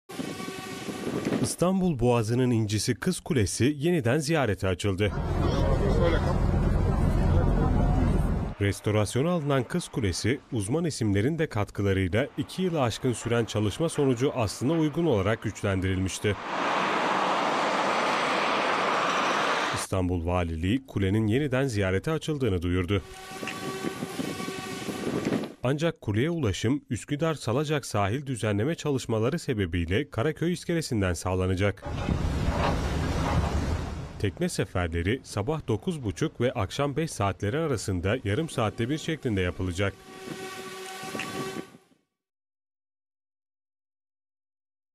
İstanbul'un simge yapılarından Kız Kulesi yeniden ziyarete açıldı
İstanbul'un simge yapılarından Kız Kulesi yeniden ziyaretçileri ağırlayacak.

Kültür ve Turizm Bakanlığı'ndan yapılan açıklamaya göre, kuleye ulaşım, Üsküdar Salacak sahil düzenleme çalışmaları sebebiyle Karaköy İskelesi'nden gerçekleştirilecek. Kuleye tekne seferleri her gün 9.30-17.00 arasında yarım saatte bir olacak şekilde gerçekleştirilecek.